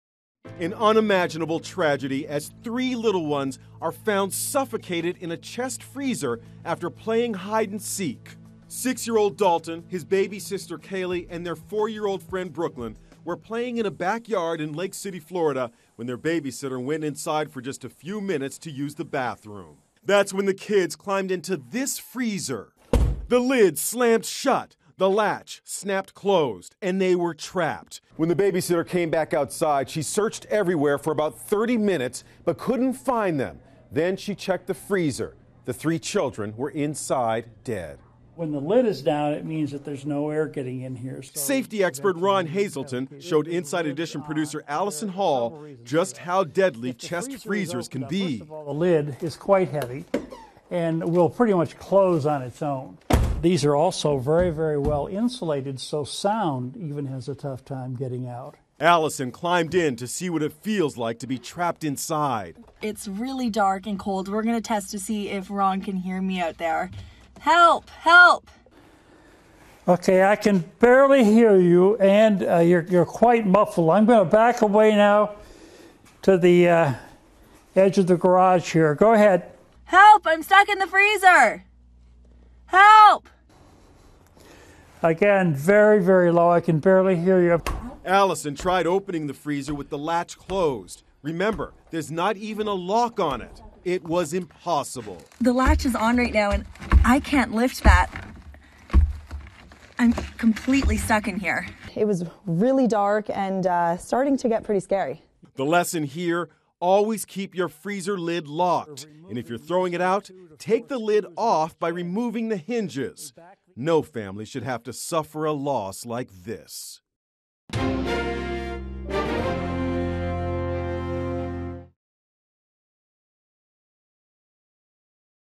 What It’s Like Being Trapped Inside a Chest Freezer
The recent deaths of three Florida children who got trapped in a chest freezer have put a spotlight on how dangerous the appliances can be. A 1-year-old, 4-year-old and 6-year-old were playing Sunday when they apparently climbed inside a freezer in the yard accidentally, the Suwannee County Sheriff’s Office said. When they were found later, they were dead, having accidentally locked themselves inside and suffocated. Inside Edition spoke with a safety expert about how to keep your family safe.